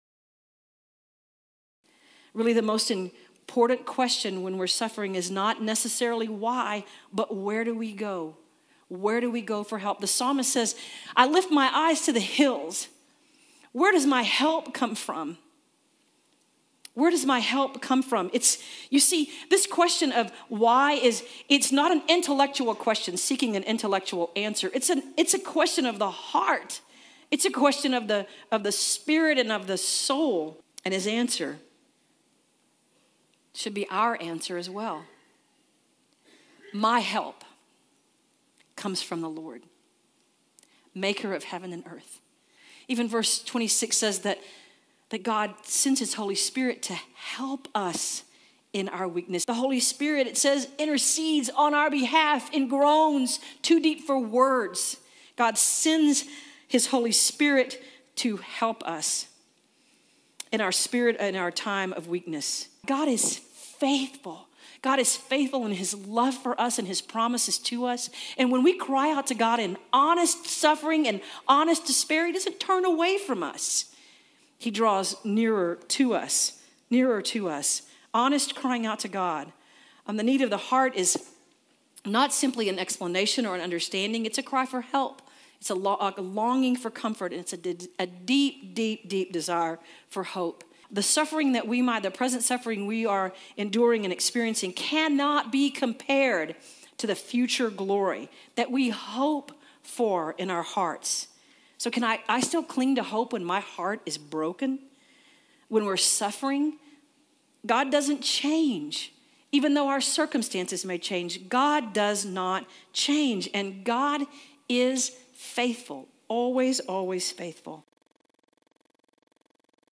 The U: University United Methodist Church Contemporary Worship Sermon Excerpt, April 3, 2016
“God is the one who won’t give up on us.” Take two with Pastor Laurinda Kwiatkowski as she uses Romans 8:18-28 in her sermon "When You're Suffering."

Watch us stream worship live to your device Sundays @ 11:00 am.